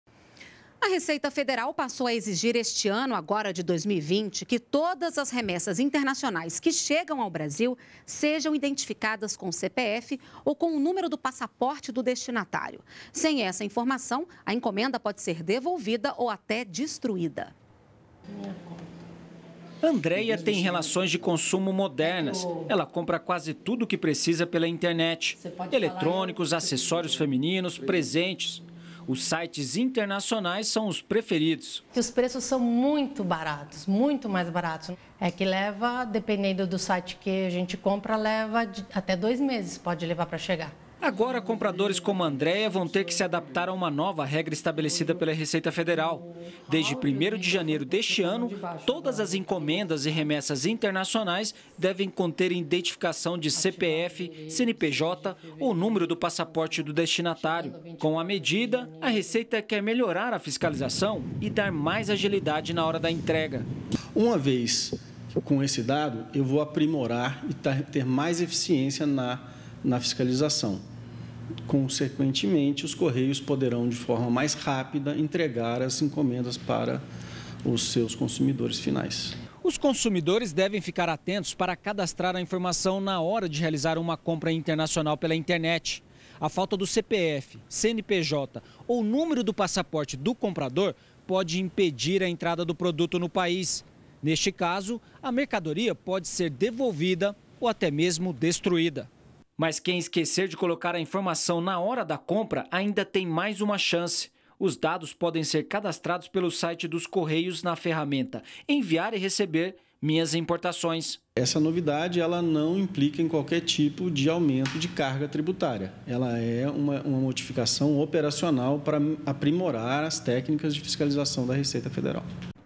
Compras internacionais devem ter CPF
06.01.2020 - Receita Federal passa a exigir identificação do CPF ou CNPJ ou Número do Passaporte do destinatário nas encomendas e remessas internacionais.